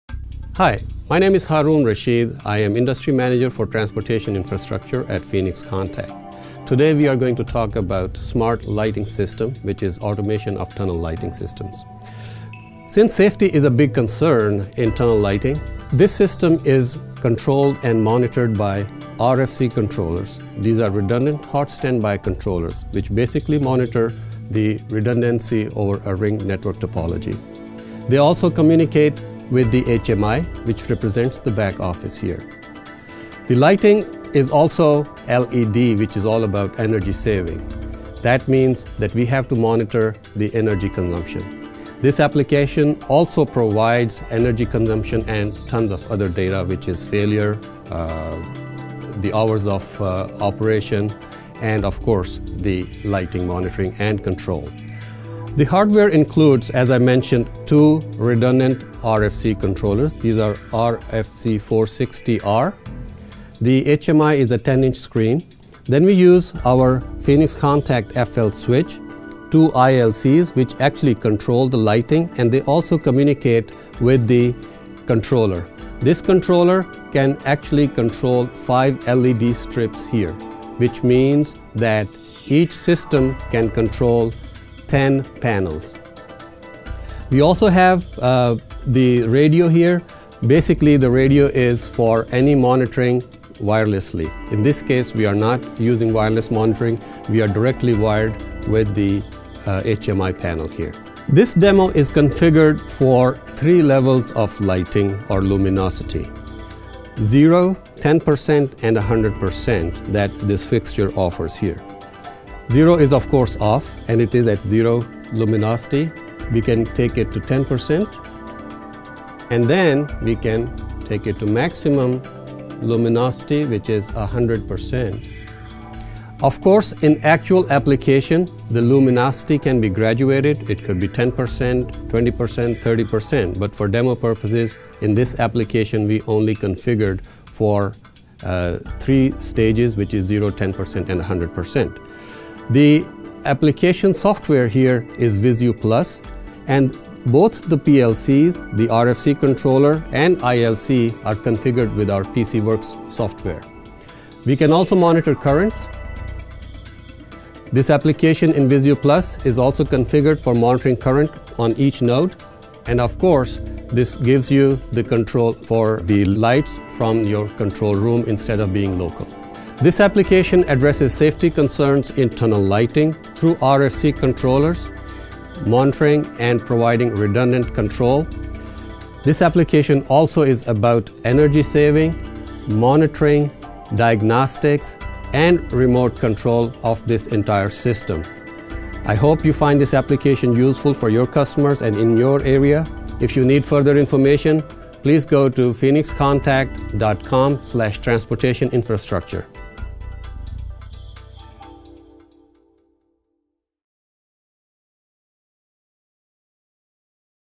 Infrastructure lighting controls for automation that will reduce energy costs - Phoenix Contact
A video description of an application that illustrates a Phoenix Contact total solution for greatly reducing the cost of lighting tunnels, bridges, parking areas and buildings. The application is described and a technical product description follows.